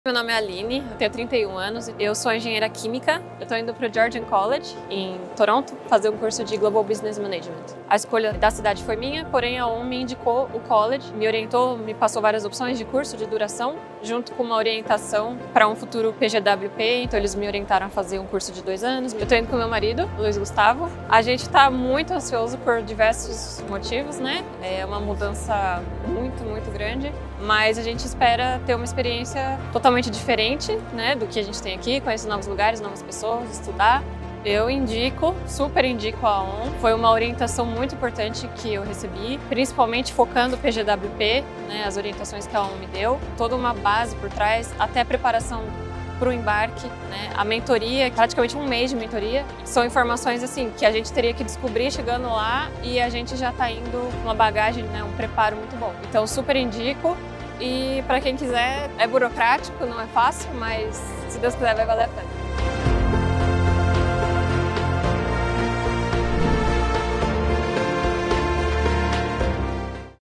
ALINE CRUZ, ESTUDANTE DO GEORGIAN@ILAC
Conheça o Aline, estudante do @GeorgiancollegeCa at @iloveilac e saiba como a On Education a ajudou, juntamente com sua família, a realizar o sonho de morar no Canadá!

Fale com um representante oficial dos principais Colleges Públicos e Privados do Canadá